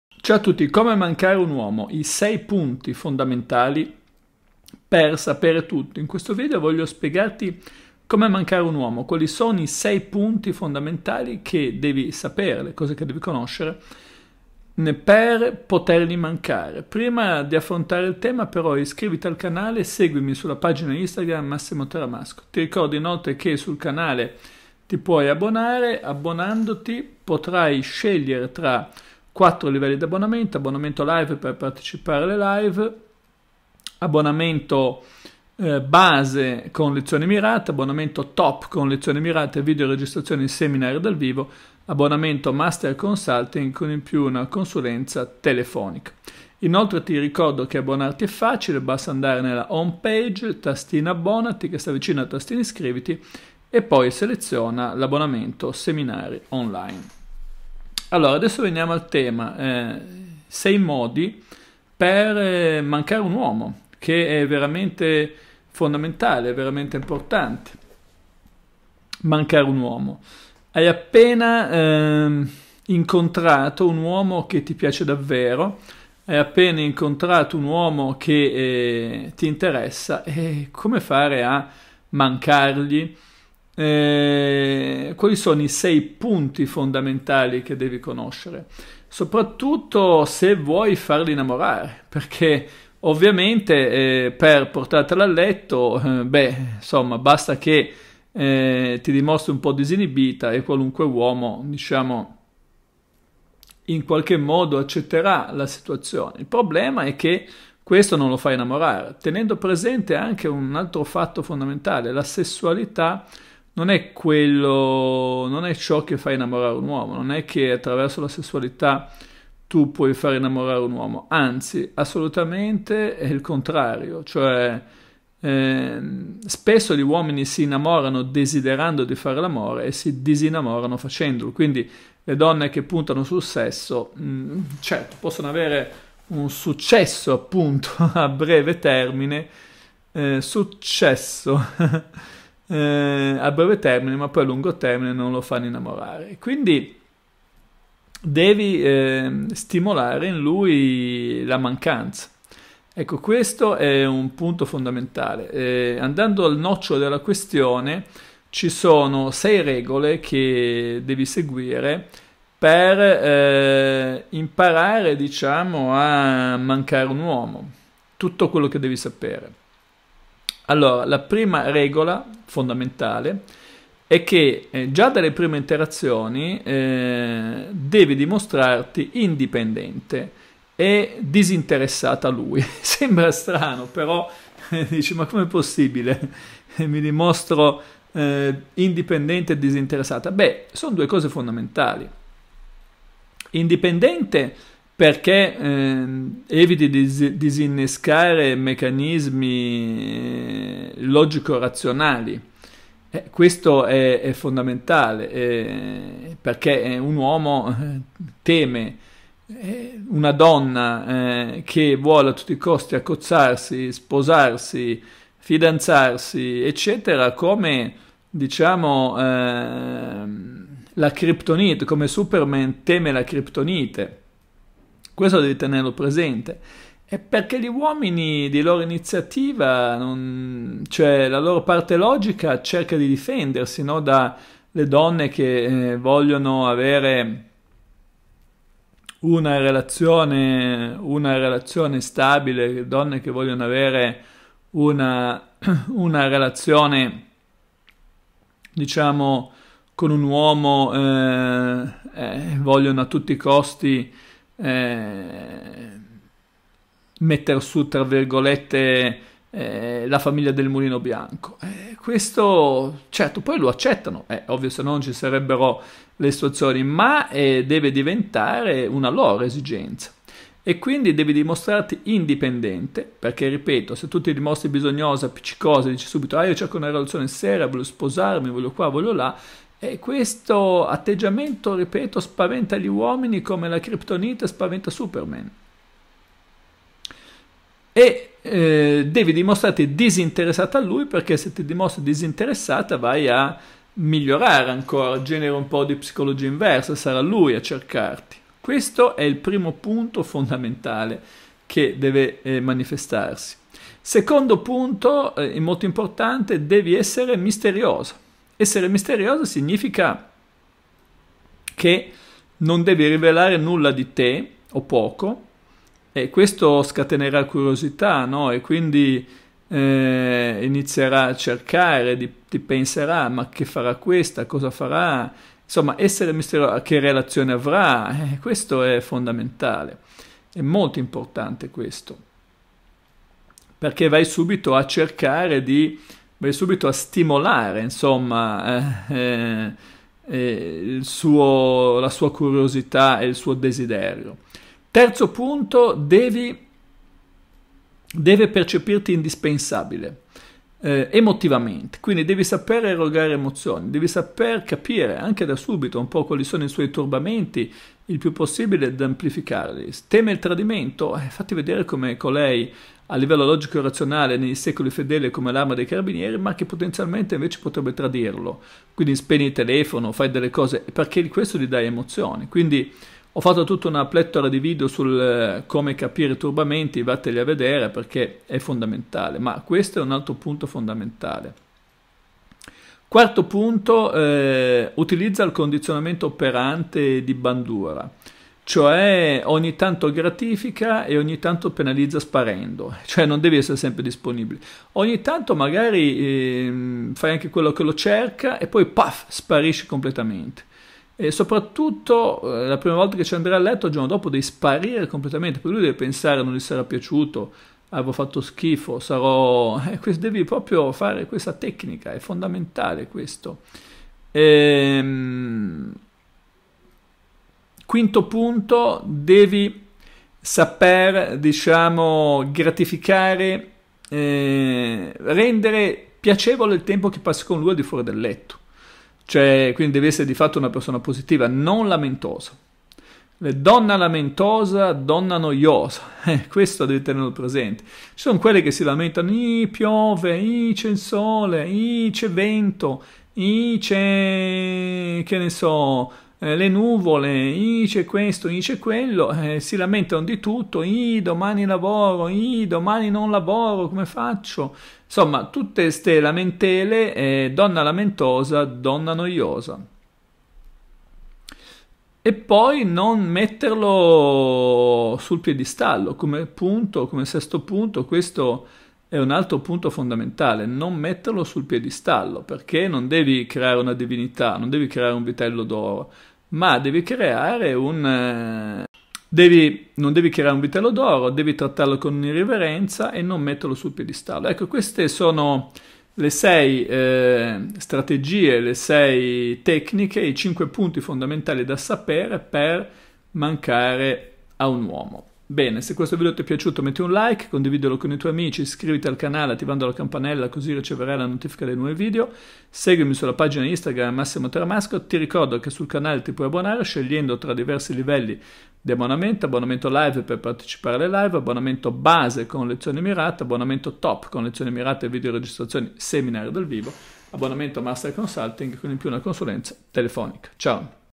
Come mancare a un uomo: i 6 punti fondamentali per sapere tutto!
In questo video ti spiego Come mancare a un uomo: i 6 punti fondamentali per sapere tutto!